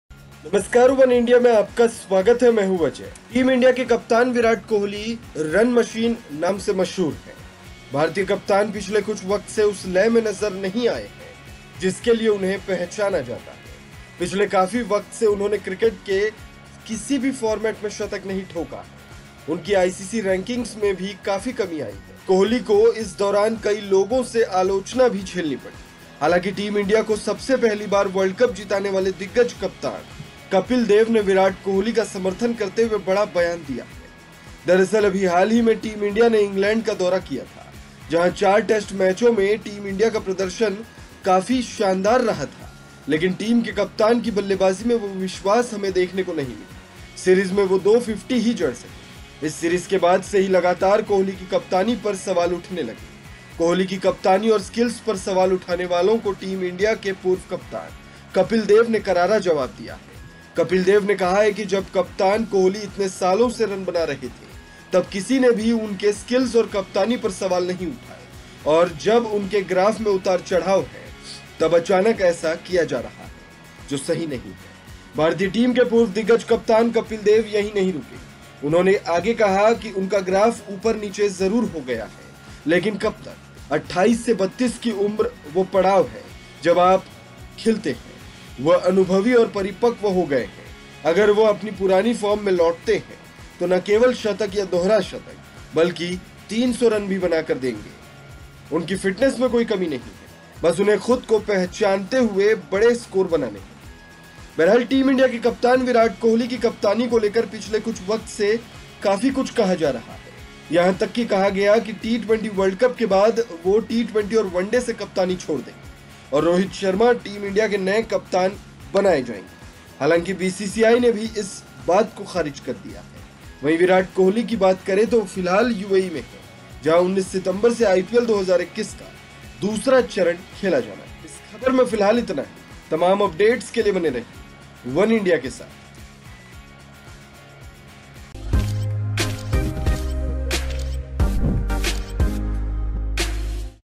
Kapil Dev ने Virat Kohli का किया बचाव, कहा- एक बार फॉर्म में लौटे तो ठोकेंगे 300 | Oneindia Sports
Team India captain Virat Kohli is popularly known as Run Machine. The Indian captain has not been seen in the rhythm for which he is recognized for some time now. For a long time, he has not scored a century in any format of cricket, his ICC rankings have also come down. Kohli had to face criticism from many people during this time, although the legendary captain Kapil Dev, who won the World Cup for the first time to Team India, has given a big statement supporting Virat Kohli.

Team India के कप्तान Virat Kohli Run Machine नाम से मशहूर हैं। भारतीय कप्तान पिछले कुछ वक्त से उस लय में नज़र नहीं आये हैं जिसके लिए उन्हें पहचाना जाता है। पिछले काफी वक्त से उन्होंने क्रिकेट के किसी भी फॉर्मेट में शतक नहीं ठोका है, उनकी ICC Rankings में भी कमी आयी है। Kohli को इस दौरान कई लोगों से आलोचना भी झेलनी पड़ी हालांकि Team India को सबसे पहली बार World Cup जीताने वाले दिग्गज कप्तान Kapil Dev ने Virat Kohli का समर्थन करते हुए बड़ा बयान दिया है।